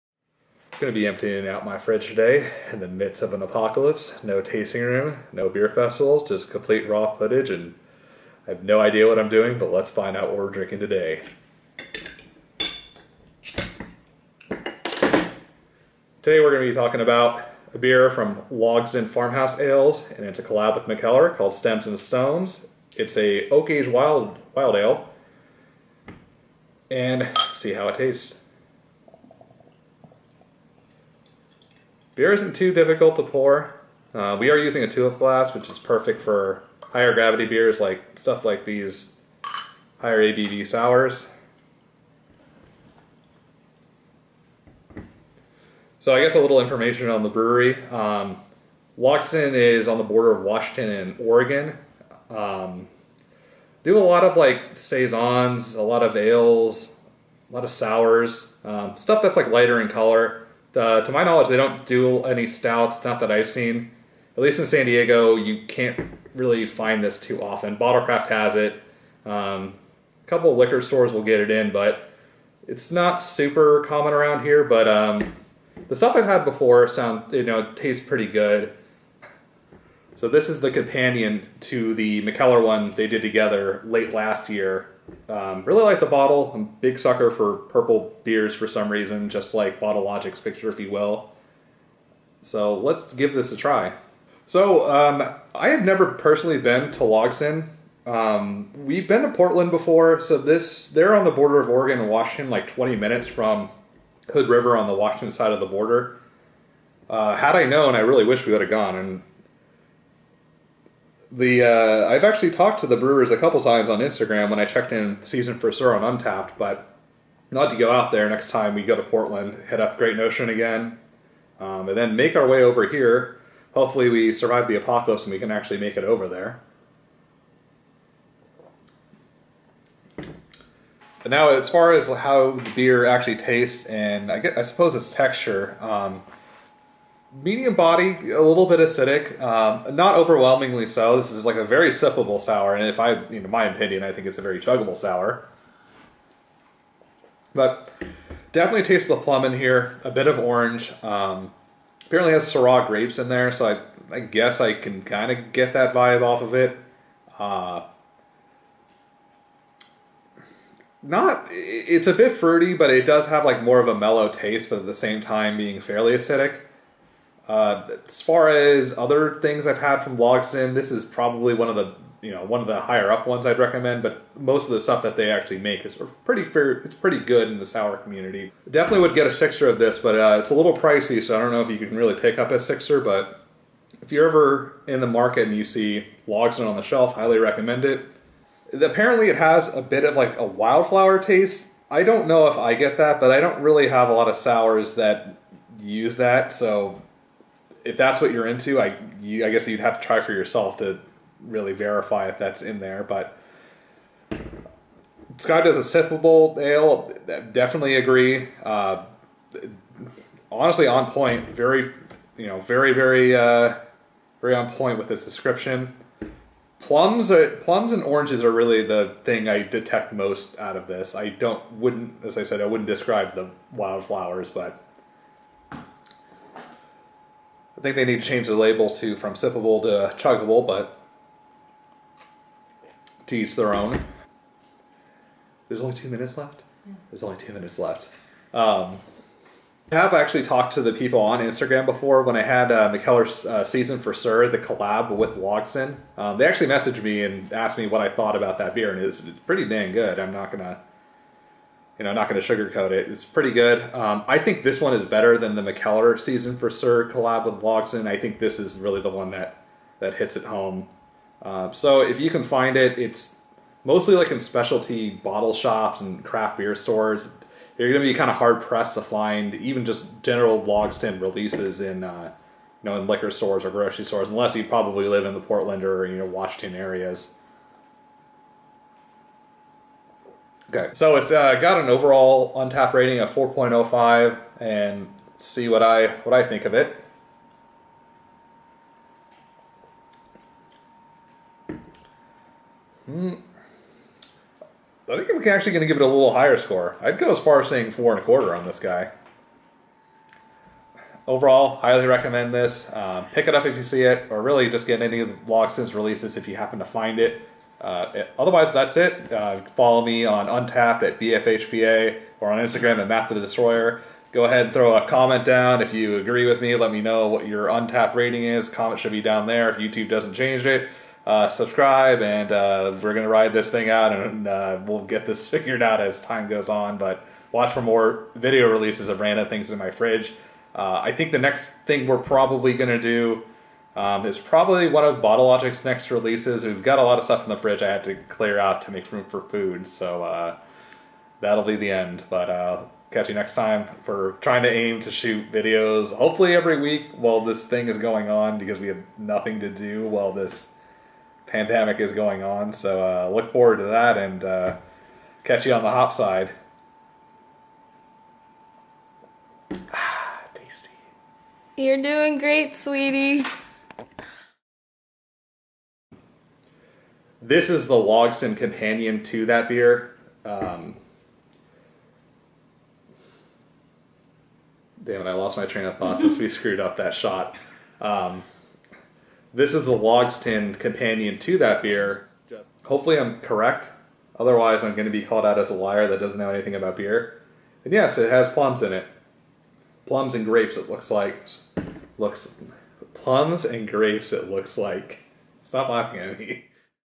What's in My Fridge?: Logsdon Farmhouse Ales / Mikkeller Stems & Stones
What's in My Fridge (Episode 1):

Logsdon Farmhouse Ales / Mikkeller - Stems & Stones - Oak Aged Wild Ale

Untappd Average Rating: 4.05/5 (As of March 2020)